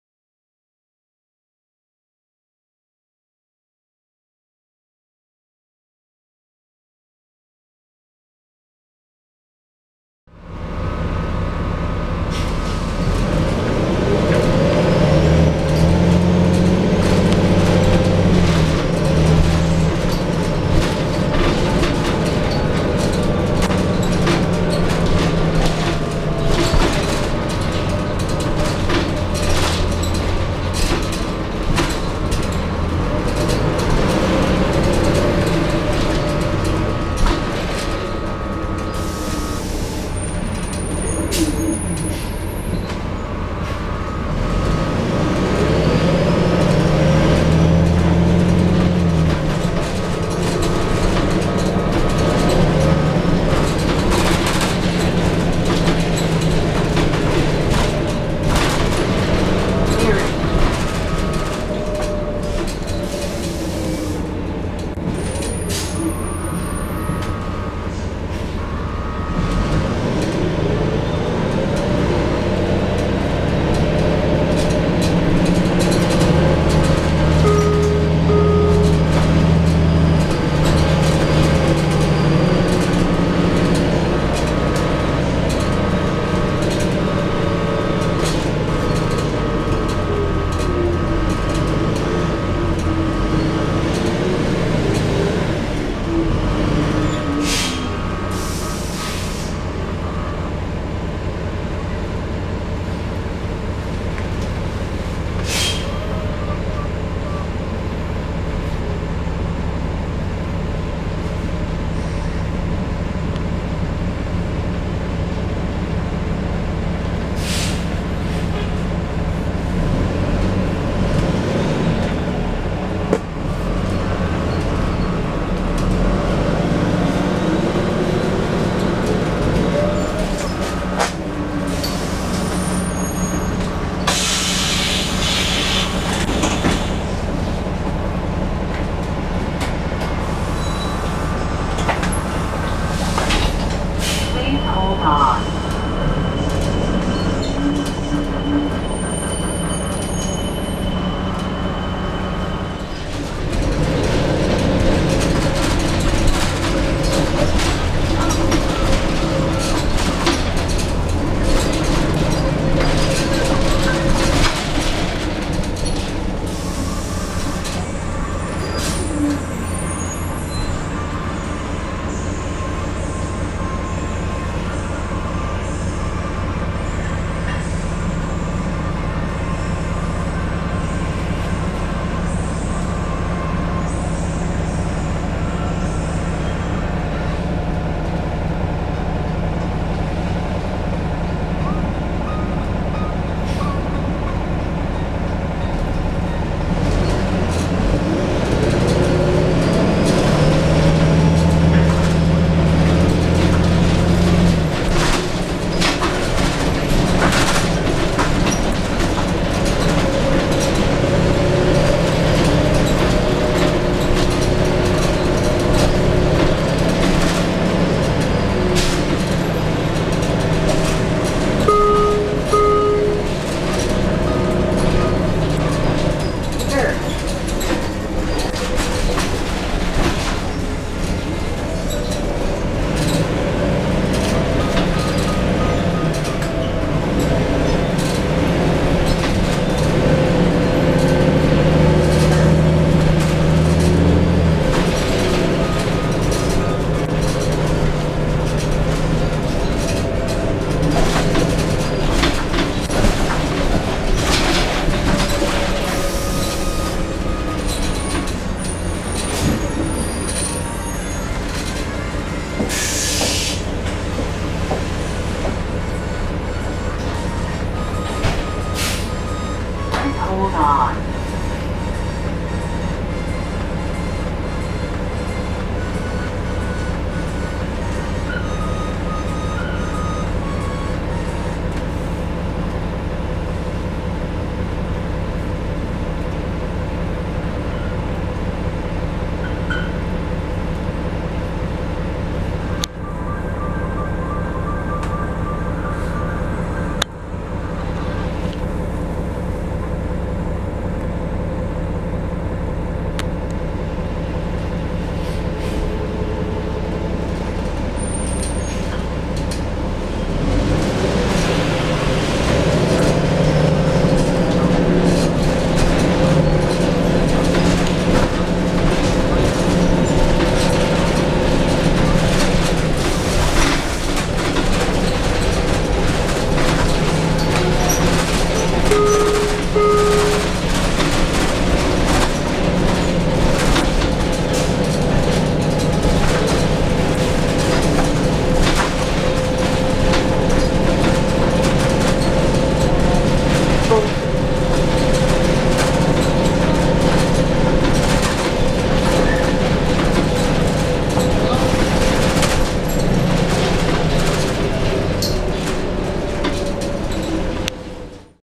SF Muni 2000 Neoplan AN440 8178 on route 43
Here is a short ride on the 43. I was glad I got 8178. A little more than two months ago, I got this bus on the same route. I wasn't too crazy about. Now, it's really bassy! Hope you enjoy this short clip!

Cummins ISM
Allison B500R

Division: Kirkland

UpDownAcross38 Films 2018